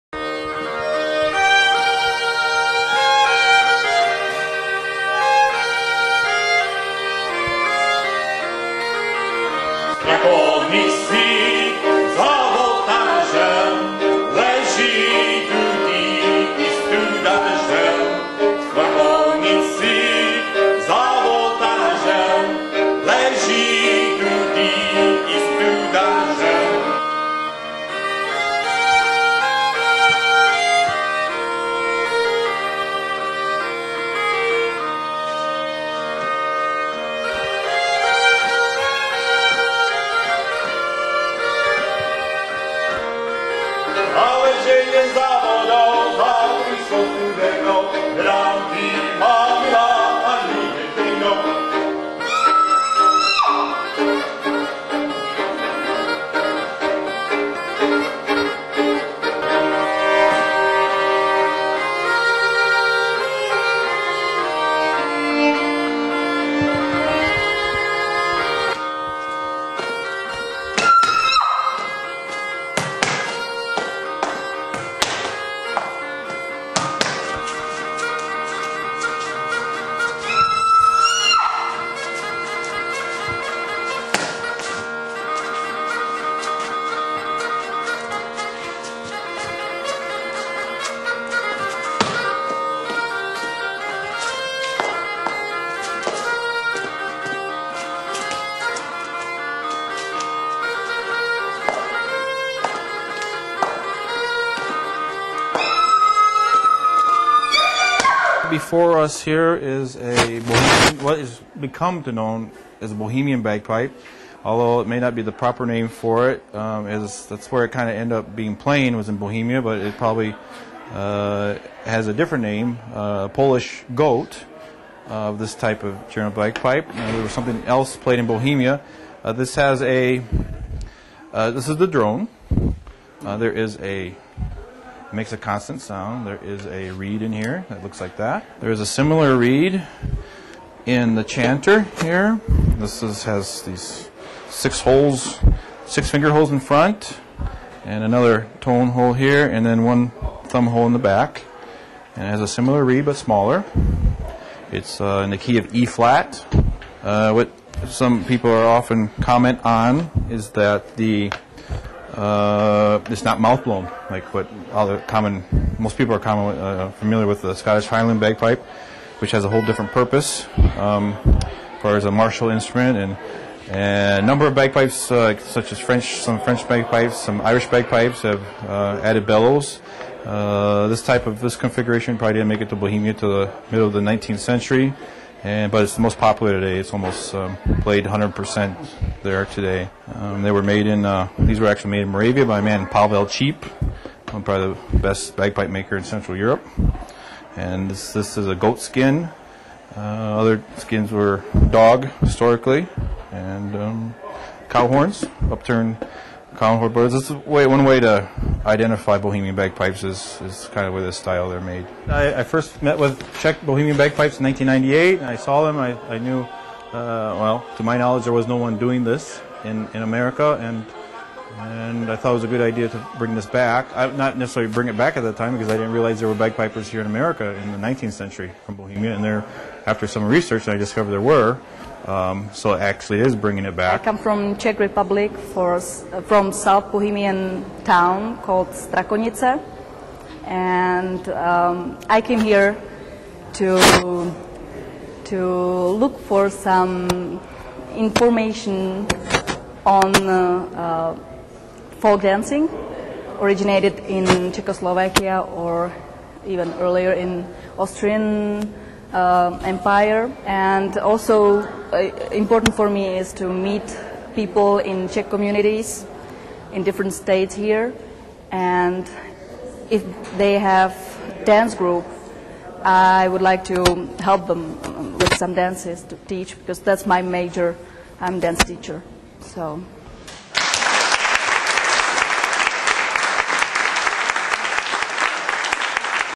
Czech bagpipes at National Czech & Slovak Museum
Czech-American bagpiper Michael Cwach and Czech violinist and folk dancer Alice Janatova brought their "Call of Dudy" tour to the National Czech & Slovak Museum & Library in Cedar Rapids, Iowa, on Sunday, January 6, 2008. The presentation included playing and singing traditional Czech folk songs, dance, and a presentation on the history of the Czech bagpipes. Also included was a screening of the documentary "Call of Dudy: Bohemian Bagpipes Across Borders." GazetteOnline video by Richard Pratt.